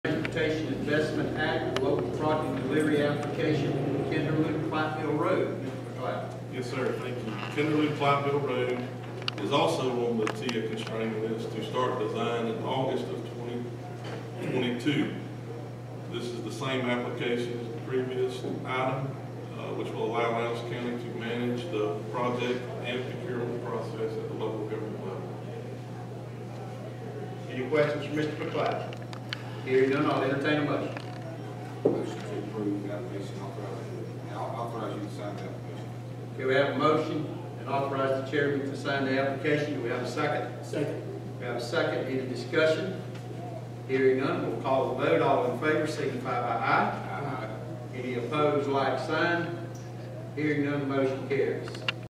6b.Transportation Investment Act (TIA) Local Project Kinderlou-Clyattville Rd@LCC Regular 2022-02-08
Unanimously voted down: Miller Bridge Road subdivision @ LCC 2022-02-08
Lowndes County Commission Regular Session, Valdosta, Lowndes County, Georgia.